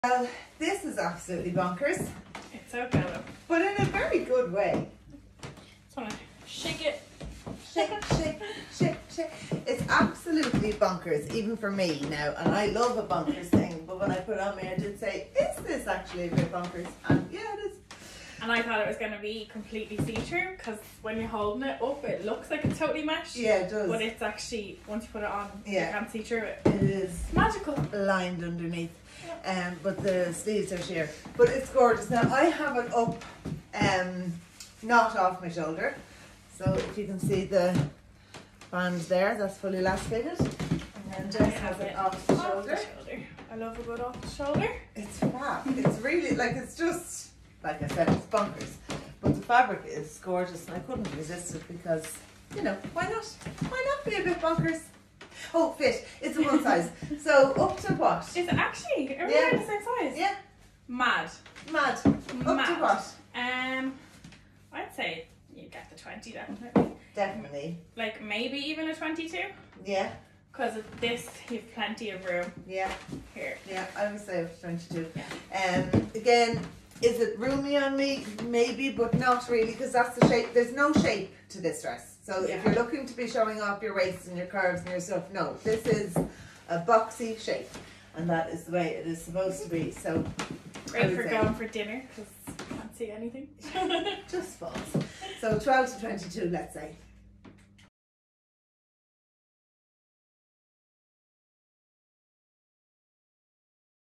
Bella Dress
Sizing and Fit:
This fun dress is a show stopper can be worn on or off the shoulder and is a great one to have over Christmas. It is a one size and will fit from a 12 to a 22.